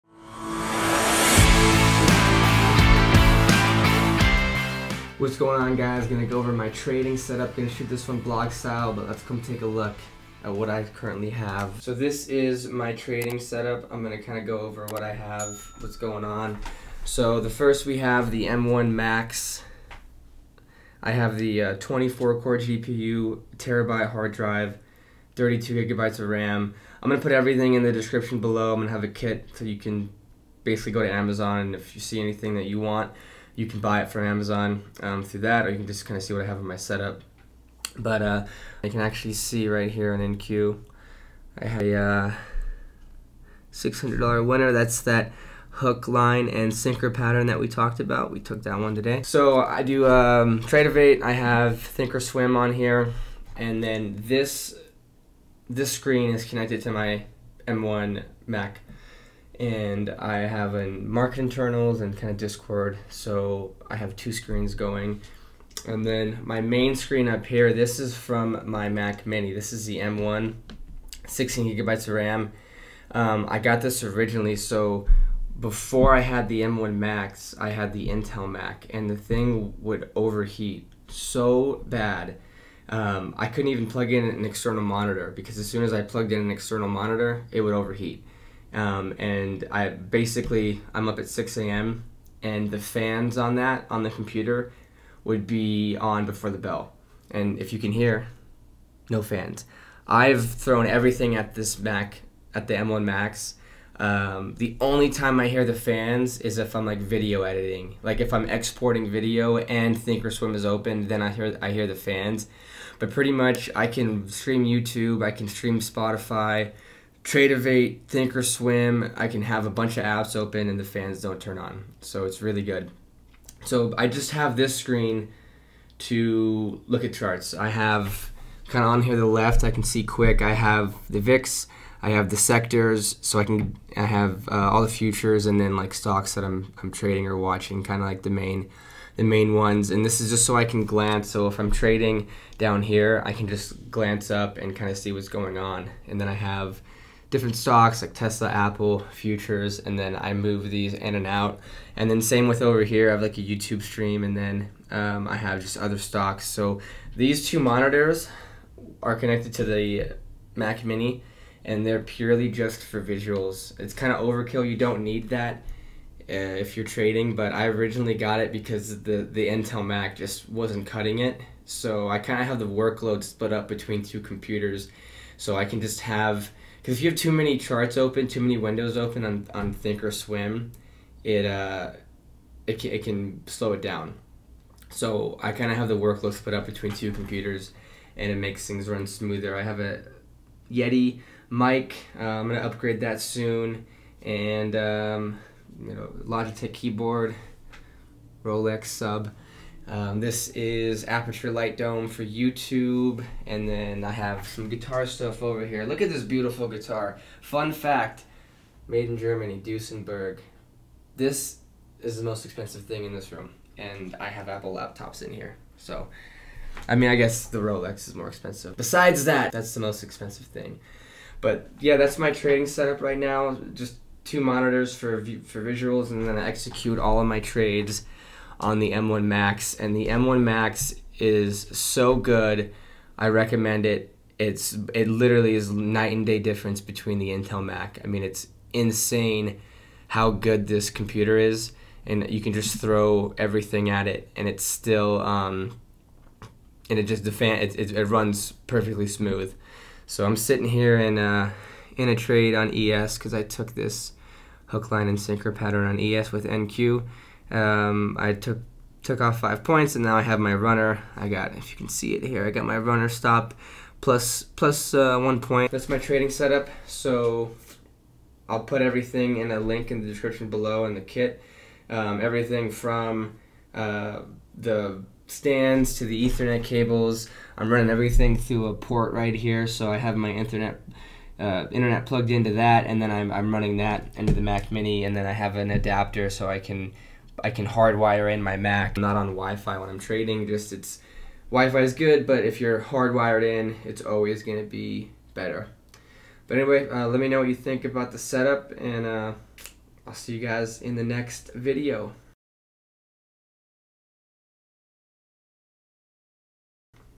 My 2023 Trading Set Up // Why the MacBook M1 Max is the BEST Trading Computer
Quick Blog style video of my trading set up.

16in Macbook M1 Max Specs:
1 TB SSD
32 GB Ram
24 Core GPU

M1 Mac mini Specs:
256 GB storage
16 gb ram
8 Core

Software:
Think or Swim - Stocks/options
Tradovate - Futures

4.📈 Prop Firms: BECOME A FUNDED FUTURES TRADER